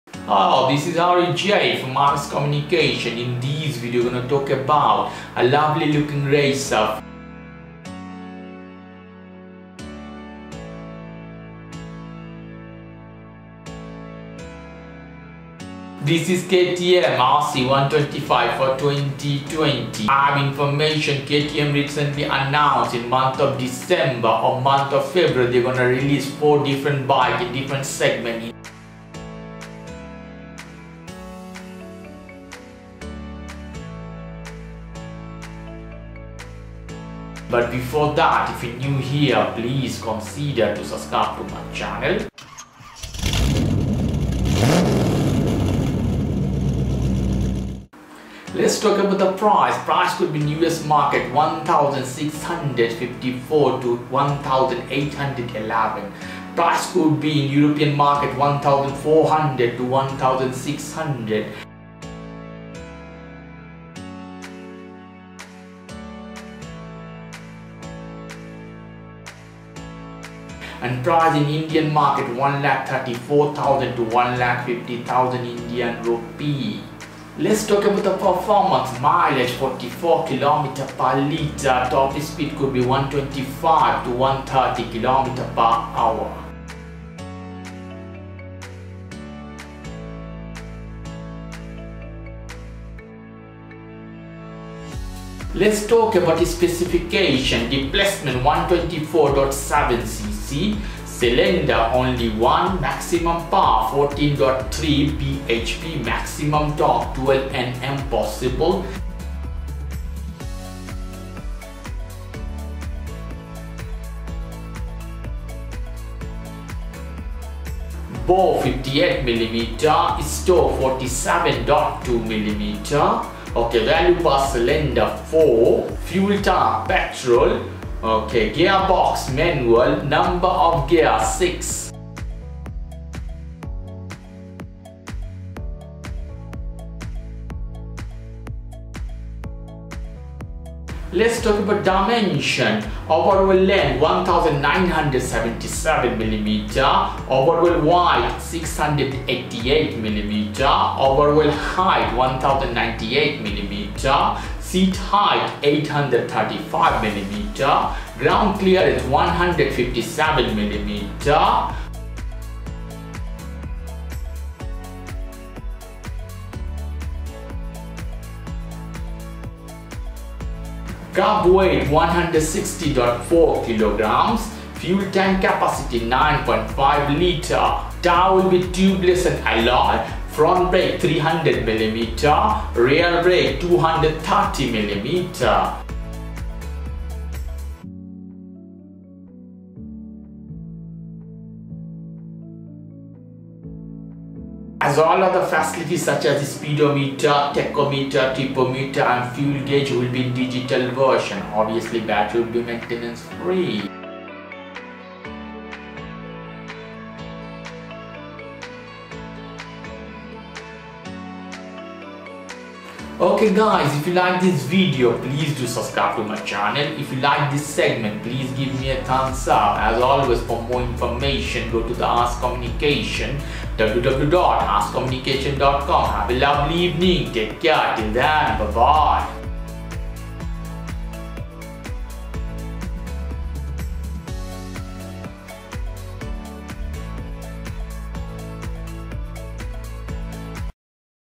KTM RC 125 2020
Specification (Ficha técnica)
Price exp.
1,655-1811 U$
1,400 -1,600 € Euro
1,34,000-1,50,00
indian Rupes

PERFORMANCE & MILEAGE
Mileage: 44 kmpl
Top Speed: 125-130 kmph

Engine Description: 1-cylinder
4-stroke engine, water-cooled
Fuel System: Fuel Injection
Displacement:124.7 cc
Maximum Power: 14.5 HP @ 9250 rpm
Maximum Torque:12 Nm @ 8000 rpm
Number of Cylinders: 1

Lubrication: Wet sump
Bore: 58 mm
Stroke:47.2 mm
Number of Gears: 6
Clutch: Wet multi-disc clutch /
mechanically operated
Primary Gear Ratio: 22:72
Secondary Gear Ratio: 14:45

Overall Length: 1977 mm
Overall Width: 688 mm
Overall Height: 1098 mm
Ground Clearance: 157 mm

Seat Height: 835 mm
Wheelbase: 1341 mm
Kerb/Wet Weight: 160.4 kg
Fuel Tank Capacity: 9.5 litres

Front Brake: 300mm Disc
Rear Brake: 230mm Disc
ABS: Yes (1-channel)
Front Tyre: 110/70 ZR17
Rear Tyre: 150/60 ZR17
Front Wheel: 17-inch
Rear Wheel: 17-inch
Wheel Type. Alloys
Tubeless Tyres:Yes
Alloy Wheels: Yes

Speedometer: Digital
Tachometer: Digital
Trip Meter: Digital
Fuel Gauge: Digital